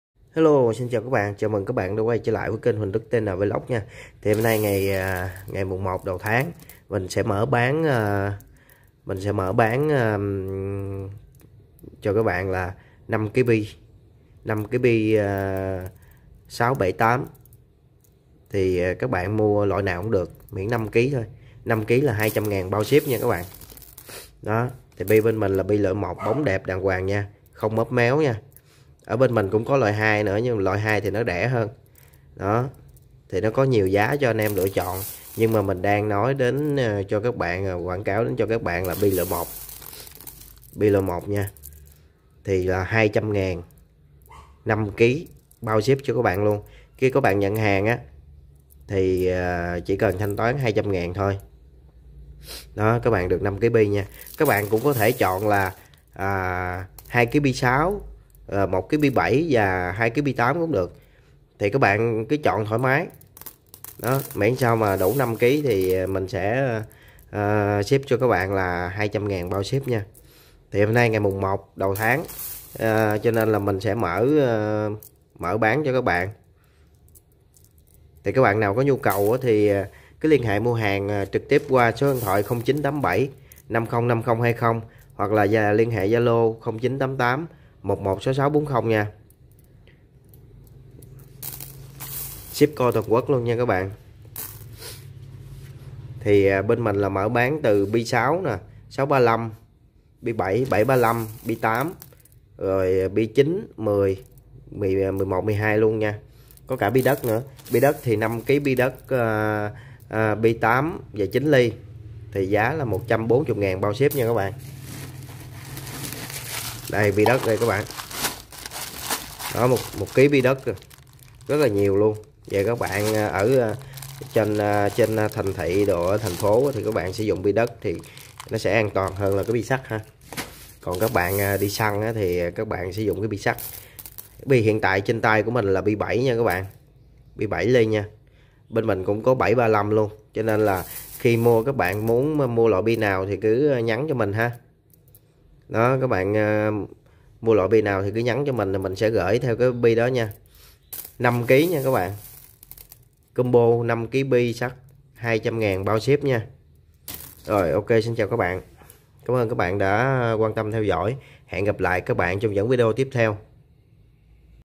Ná Cao Su: Combo 5kg bi sắt dùng bắn ná 200k bao ship toàn quốc- Huỳnh Đức TN Vlogs
hôm nay Mồng 1
nên shop mình mở bán combo 5kg bi sắt 200k bao ship.

bi 6, 635, 7,8,9,10,11,12.

bi đất 5kg 140k bao ship.

các bạn nhanh tay đặc hàng nhé.